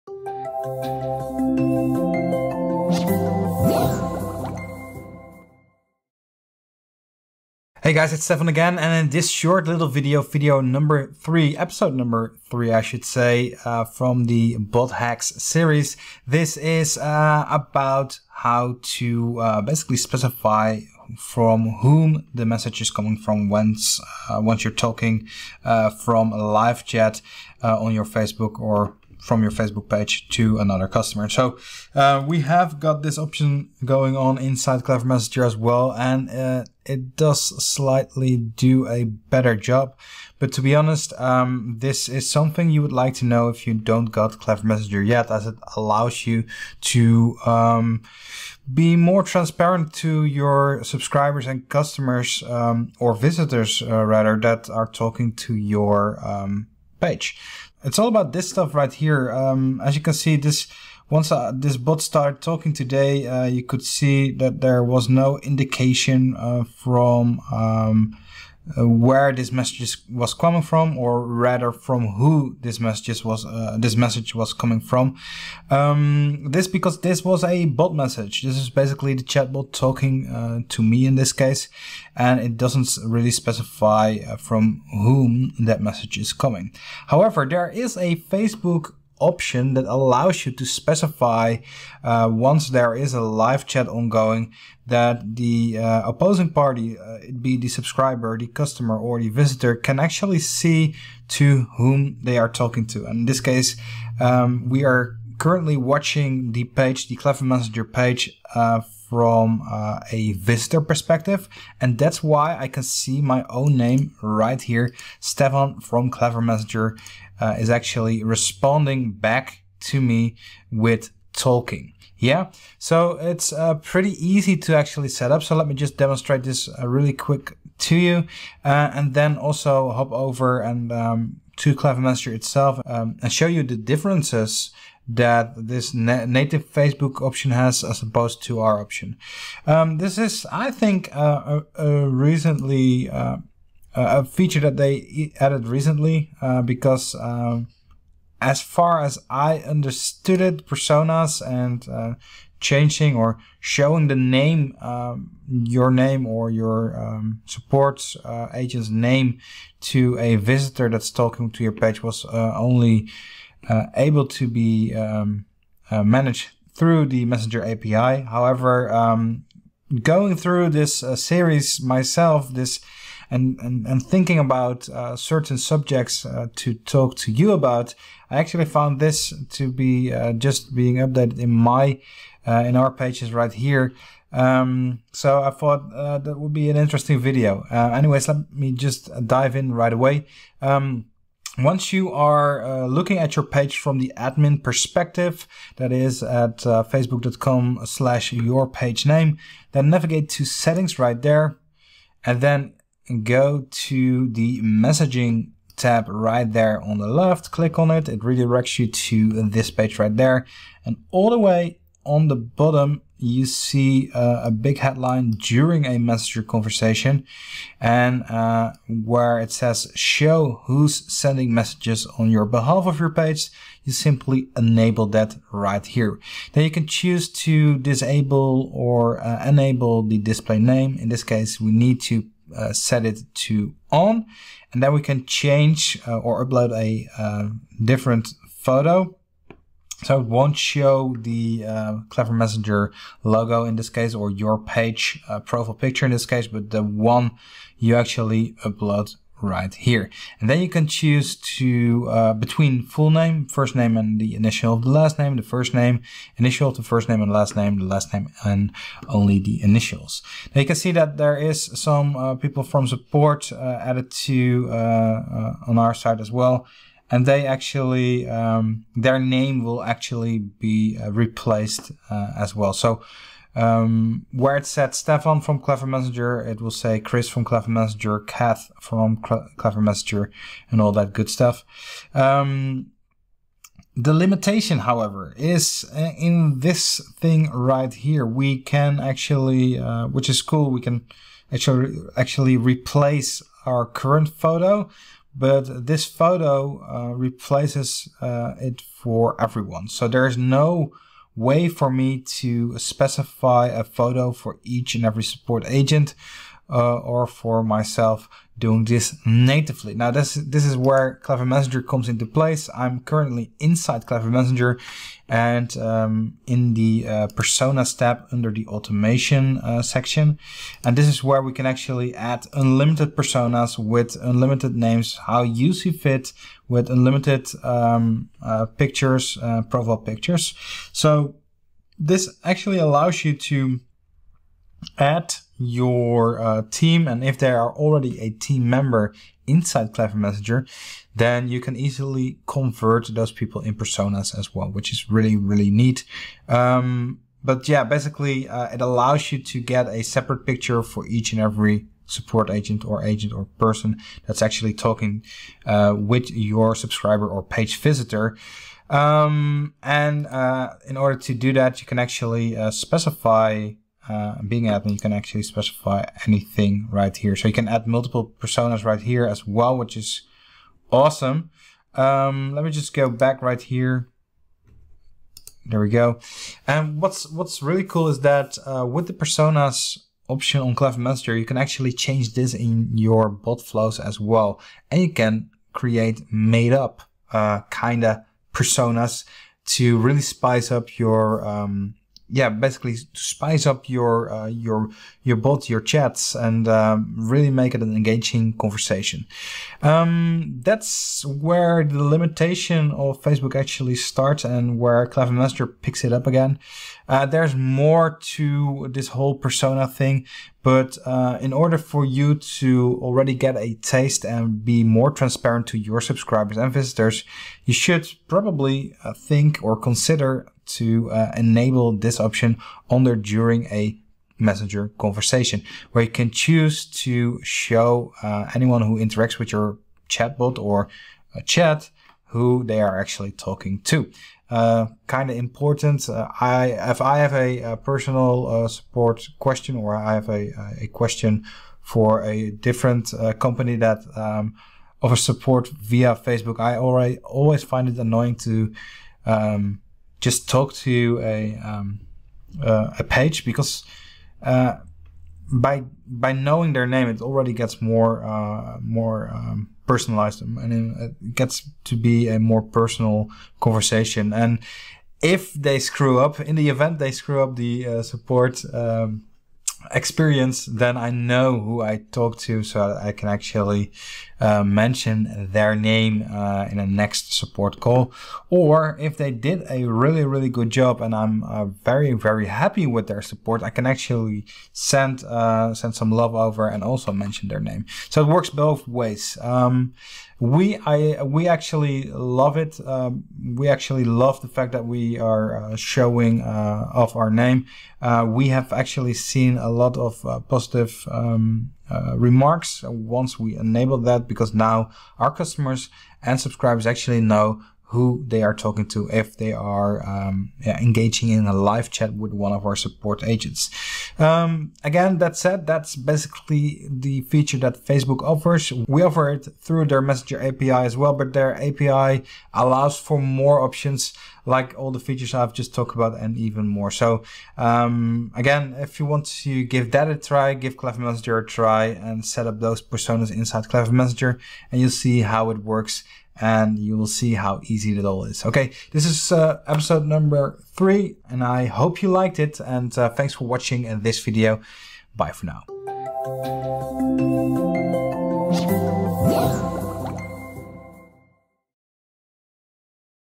Show off Names inside a Facebook Messenger Chat with Customers? (Chatbot Page Hacks - Episode 3)
With the Chatbot Page Hack Series, you discover how to optimize your Facebook pages in such a way to get more messages from your visitors and subscribers. In this episode, you learn how to show off names inside a Facebook Messenger Support chat with customers!